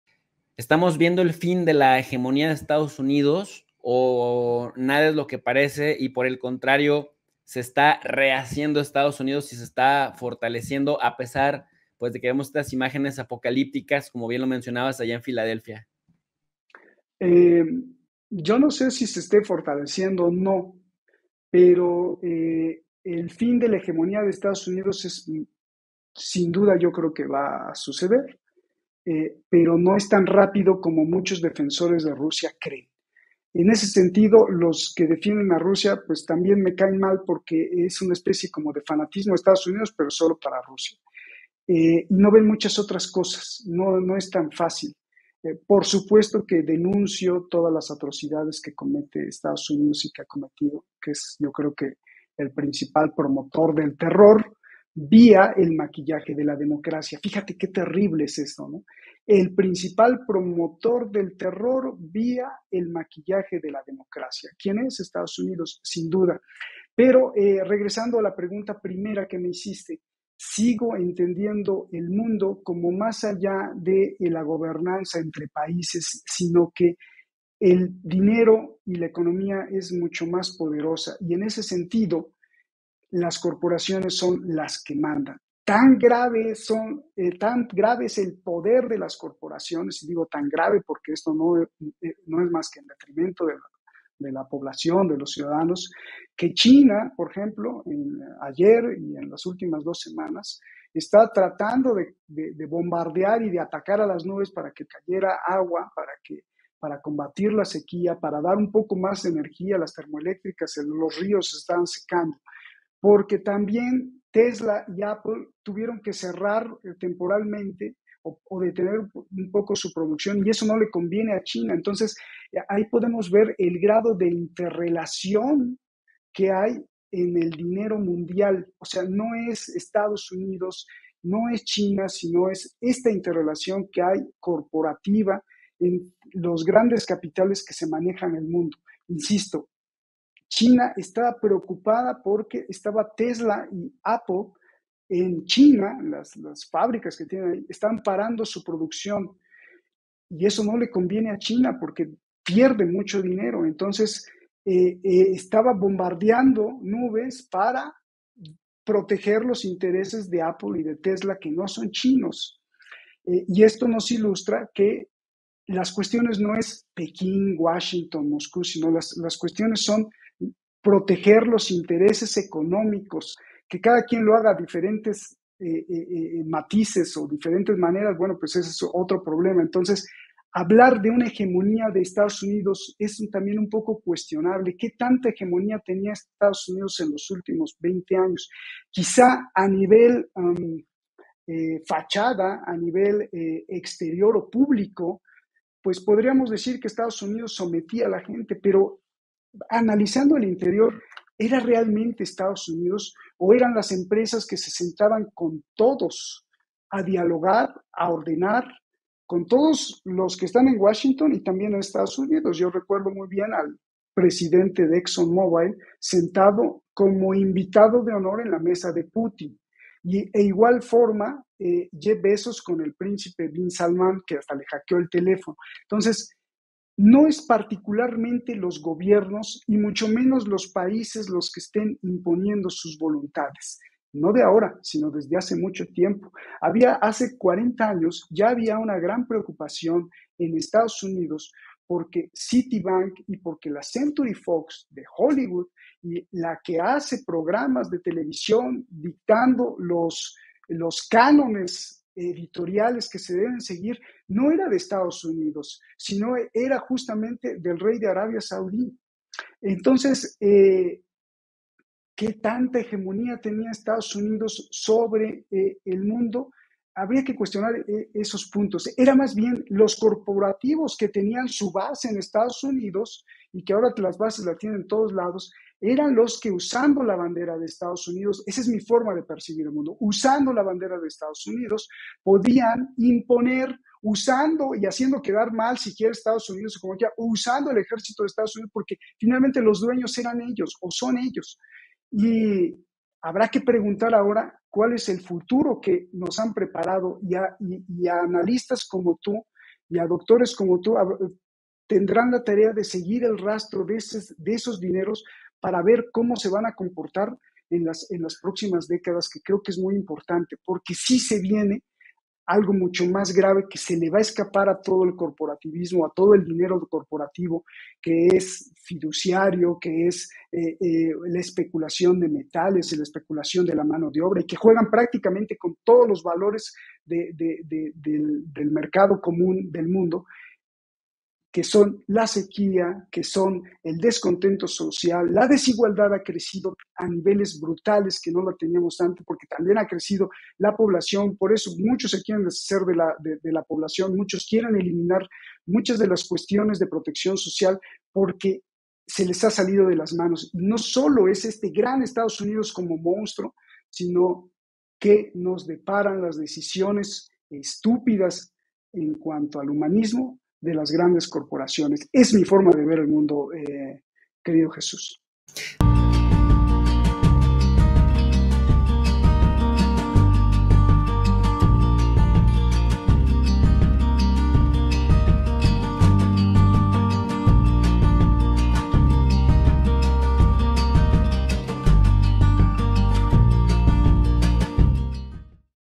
NO SON LOS GOBIERNOS, LAS CORPORACIONES SON LAS QUE MANDAN
En este video Roberto de la Madrid nos comparte quiénes son los que eligen el rumbo del mundo.